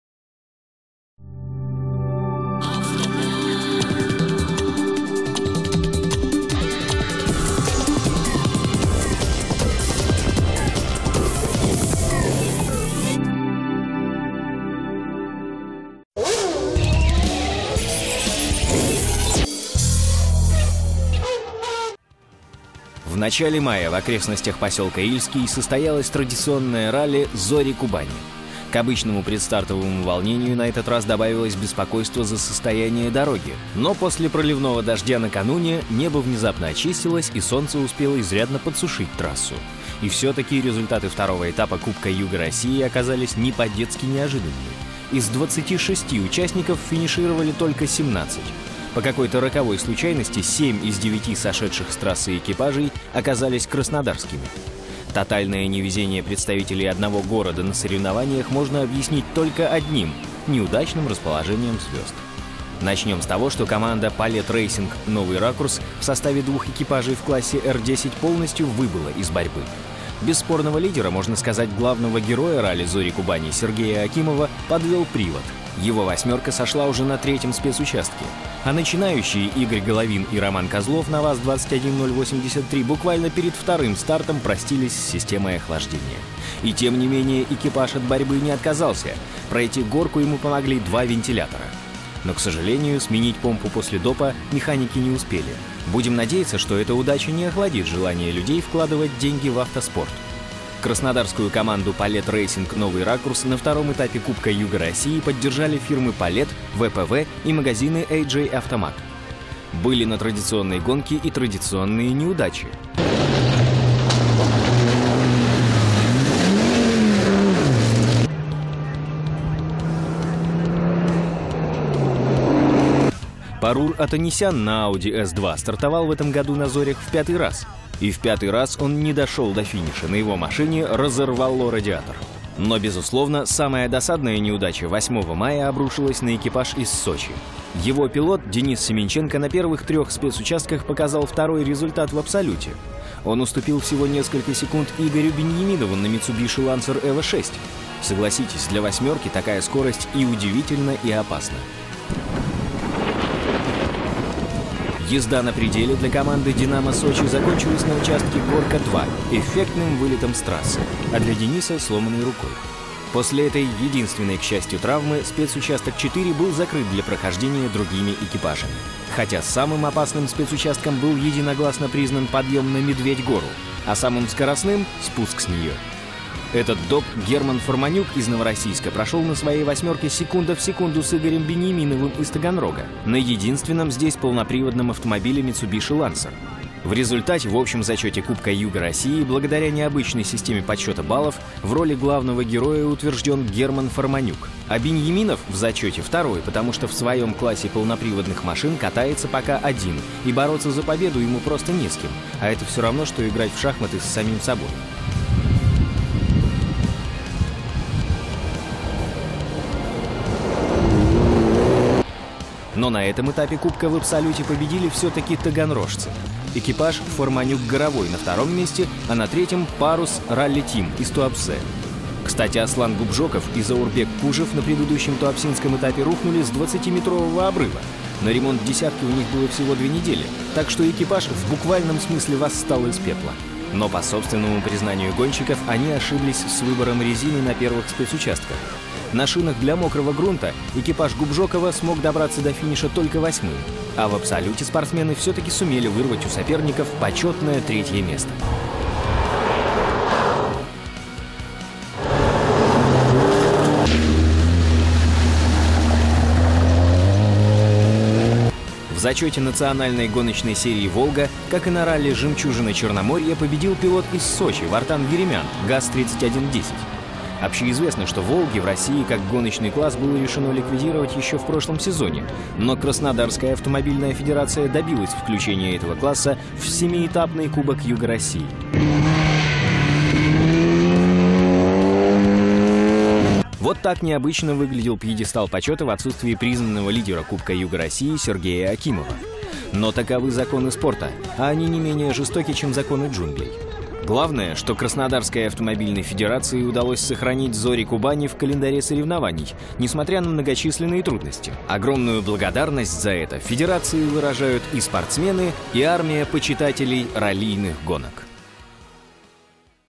Автобан Ралли "Зори Кубани 2004г."
2-ой этап кубка юга России по авторалли

Видеозапись является собственностью
ЗАО ТРК «Новый Ракурс»